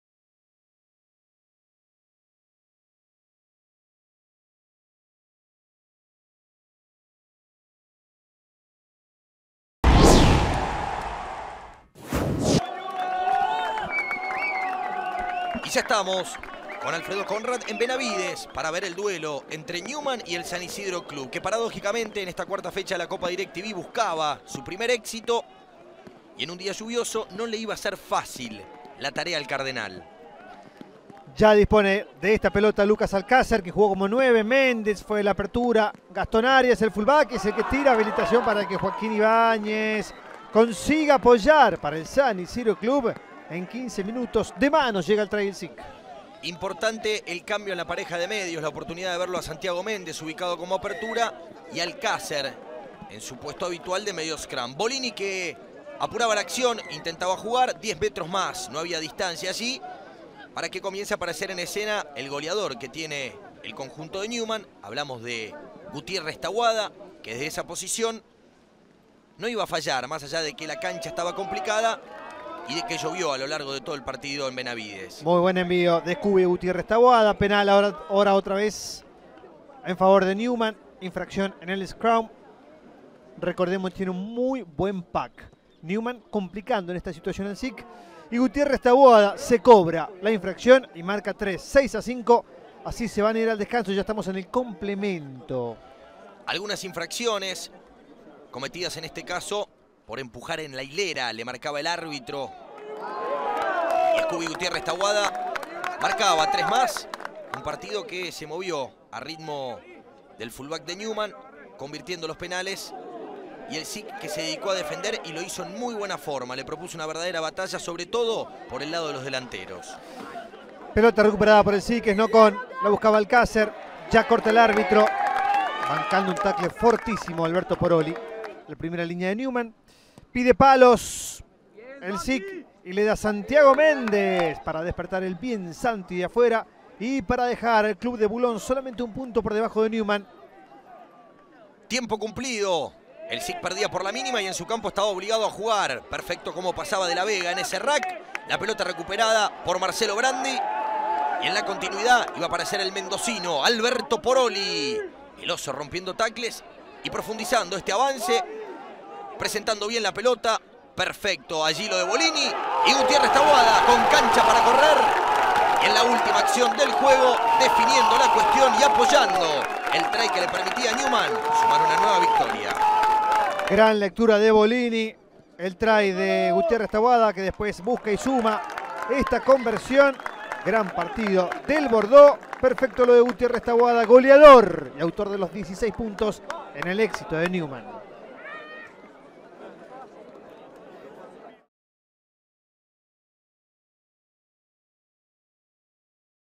SIC vs Newman - Top 14 - Resumen ESPN
Publicamos el resumen de ESPN del partido con Newman, por la cuarta fecha del top 14.